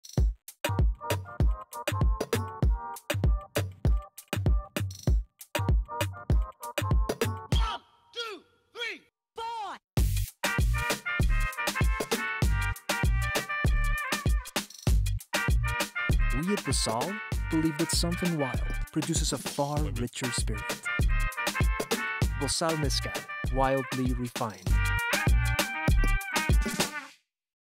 Bozal Mezcal - Wildly Refined
On the steep hillsides of Mexico, varieties of agave grow wild. Keeping with 200 year old traditions, we use an artisanal approach, offering rare mezcals with rich and savory flavors. We at Bozal believe that something wild produces a far richer spirit.